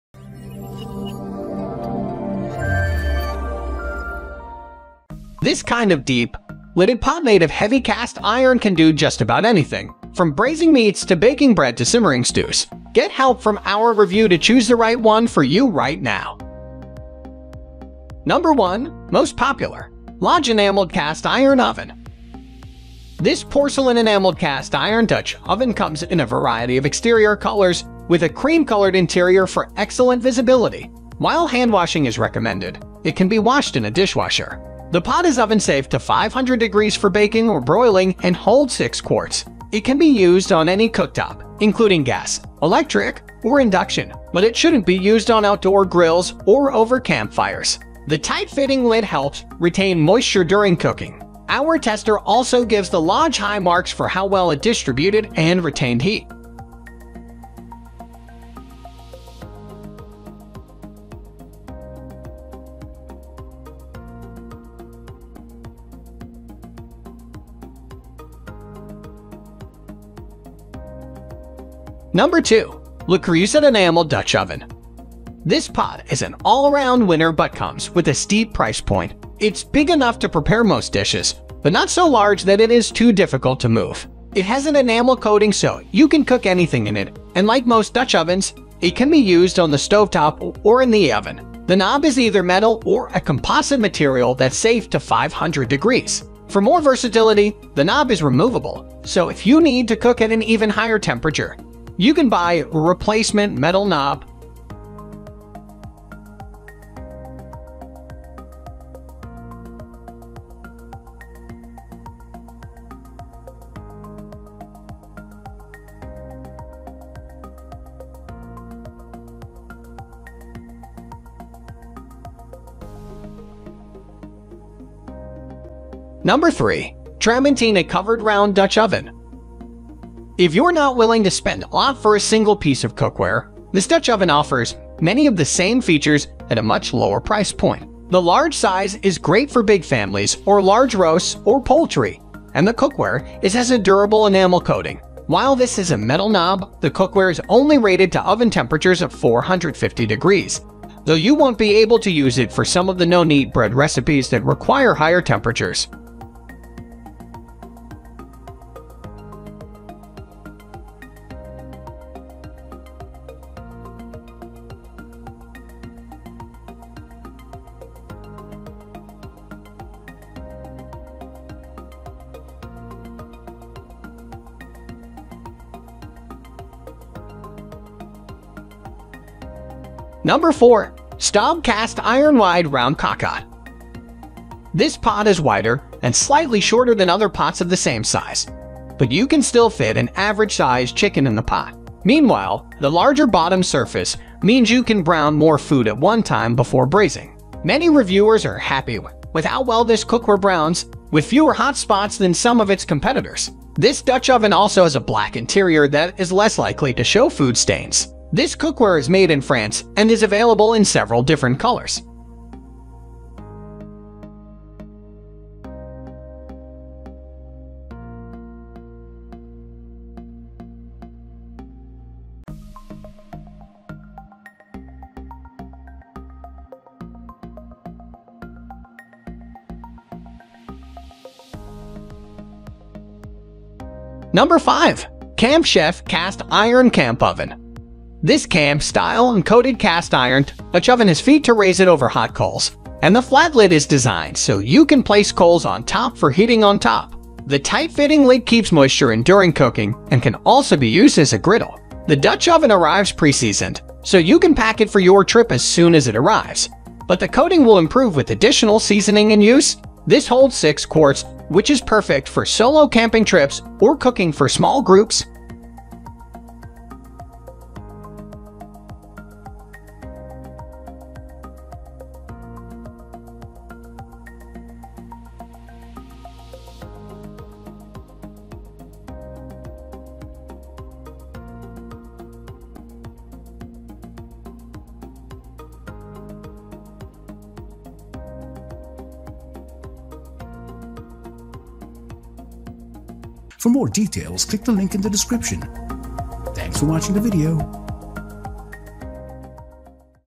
Top 5 Best Enameled Cast Iron Cookware’s Review in 2023 | for All Types of Cooking
Best enameled cast iron cookwares featured in this Video:
0:18 NO.1. Lodge Enameled Cast Iron Oven
1:12 NO.2. Le Creuset Enameled Dutch Oven
2:05 NO.3. Tramontina Covered Round Dutch Oven
3:00 NO.4. STAUB Cast Iron Wide Round Cocotte
3:54 NO.5. Camp Chef Cast Iron Camp Oven

What is an enameled cast iron cookware?

An enameled cast iron cookware is basically just a regular cast iron pan with an enamel coating. The enamel helps to protect your kitchen equipment from all sorts of unwanted things, including rust, while also adding a new element to your cooking process, as enamel cast iron is wonderful for slow cooking.